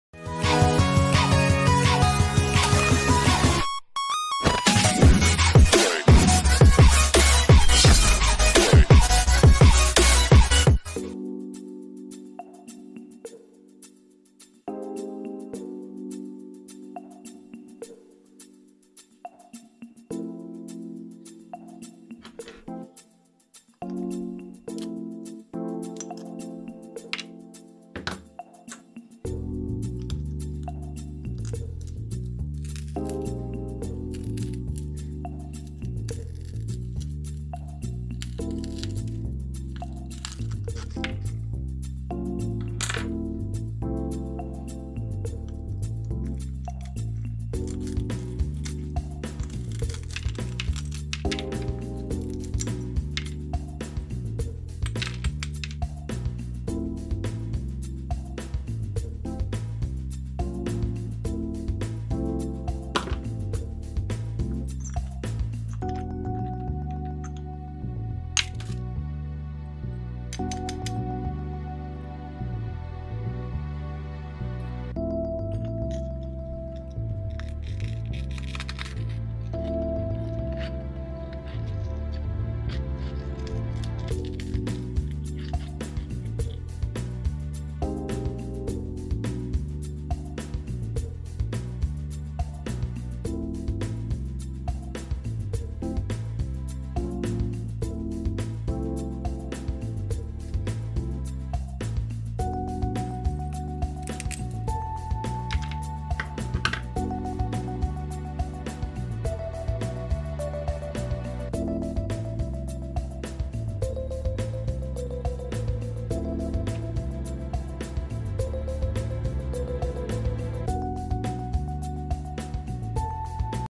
How to Empty a Light Bulb
Ciao, in questo video vi mostro come svuotare una lampadina ad incandescenza, per recuperarne la parte in vetro... La parte in vetro potrà poi essere riutilizzata come contenitore per altri progetti.

Non mi ritengo responsabile di alcun danno causato dalla riproduzione, anche parziale, della tecnica da me mostrata in questo video.